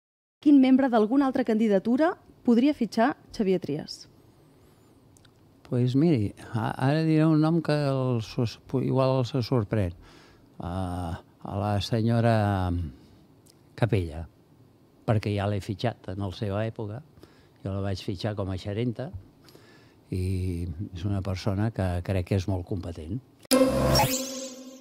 28M | Xavier Trias, candidat de Junts a l'alcaldia de Barcelona
El candidat a l'alcaldia de Barcelona, Xavier Trias (Trias per Barcelona) en una entrevista a SER Catalunya s'ha compromès a sol·licitar la Declaració de Barcelona com a zona tensionada de lloguers per limitar-ne els preus aplicant la Ley de Vivienda aprovada al Congrés. Malgrat que Junts per Catalunya va votar-hi en contra, Trias considera que ho va fer perquè "envaïa competències de la Generalitat", i ha afirmat que per rebaixar el preu de l'habitatge "totes les eines són bones".